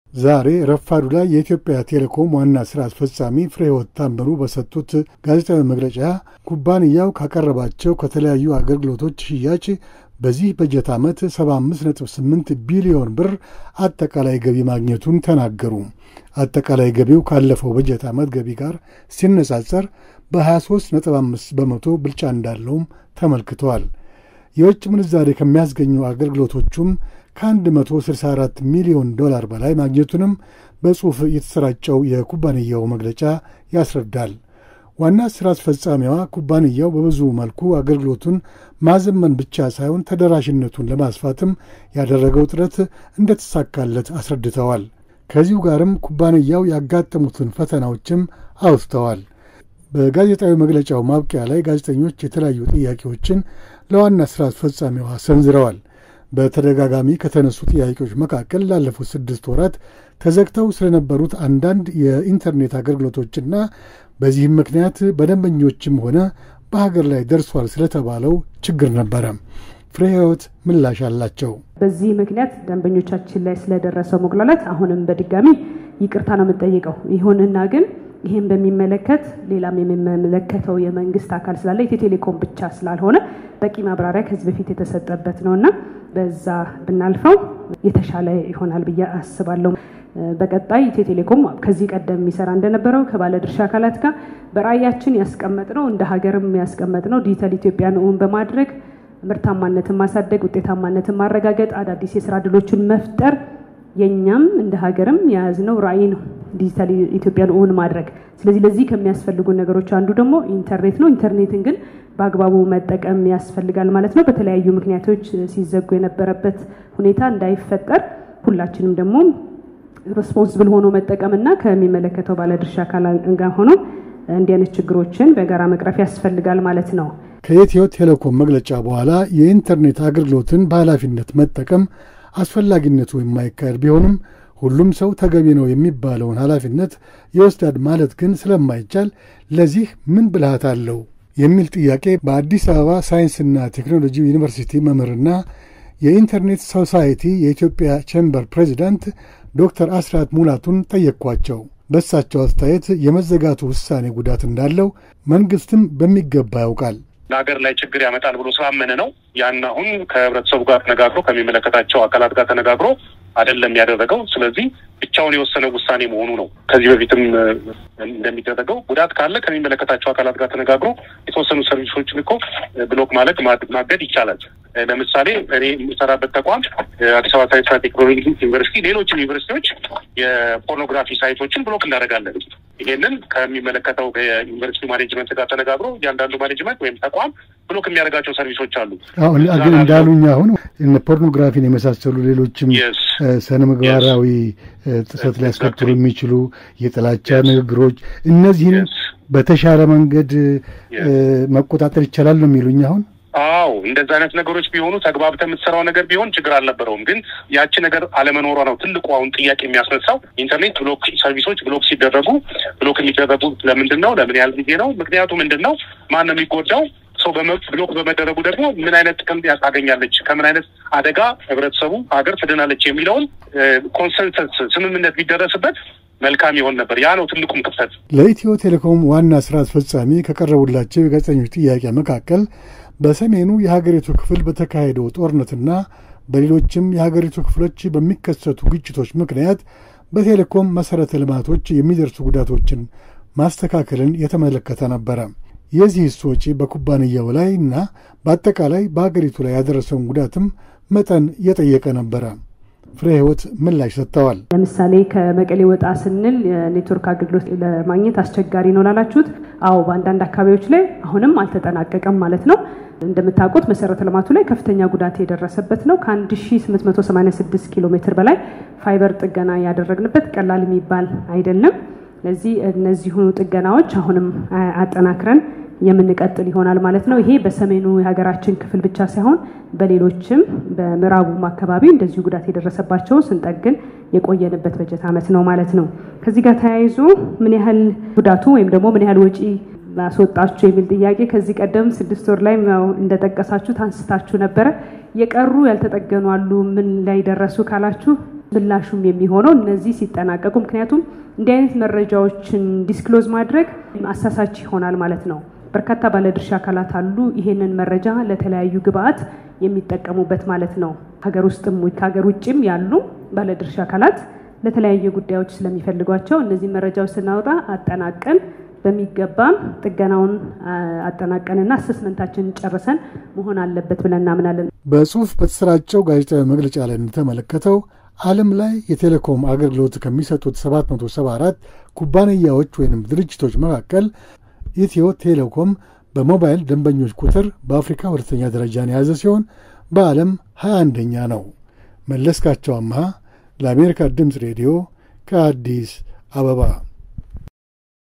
ኢትዮ ቴሌኮም በበጀት ዓመቱ ከ75 ሚሊዮን ብር በላይ ገቢ ማግኘቱን አስታወቀ
ኢትዮ ቴሌኮም በ2015 ዓ.ም. በጀት ዓመት፣ 75ነጥብ8 ቢሊዮን ብር ገቢ ማግኘቱን አስታወቀ።የበጀት ዓመቱን መጠናቀቅ አስመልክቶ፣ ዛሬ ማክሰኞ፣ ሐምሌ 11 ቀን 2015 ዓ.ም. ጋዜጣዊ መግለጫ የሰጠው ኢትዮ ቴሌኮም፣ ገቢው ካለፈው ዓመት ጋራ ሲነጻጸር፣ በ14ነጥብ5 ቢሊዮን ብር መጨመሩን አስታውቋል።

ላለፉት ስድስት ወራት፣ የማኅበራዊ ትስስር ብዙኃን መገናኛ በመዘጋቱ ምክንያት፣ በደንበኞች ላይ ለደረሰው ጉዳት እና መጉላላት፣ የኩባንያው ዋና ሥራ አስፈጻሚ ወ/ሪት ፍሬ ሕይወት አያሌው፣ በዚኹ መግለጫ አጋጣሚ ይቅርታ ጠይቀዋል።

በሰሜኑና በሌሎችም የአገሪቱ ክፍሎች፣ በቴሌኮም መሠረተ ልማቶች ላይ የደረሰውን ጉዳት ሙሉ ለሙሉ ለመጠገን፣ አሁንም እየተሠራ እንደኾነ ኢትዮ ቴሌኮም አስታውቋል።

ሙሉ ዘገባውን ከተያያዘው ፋይል ይከታተሉ።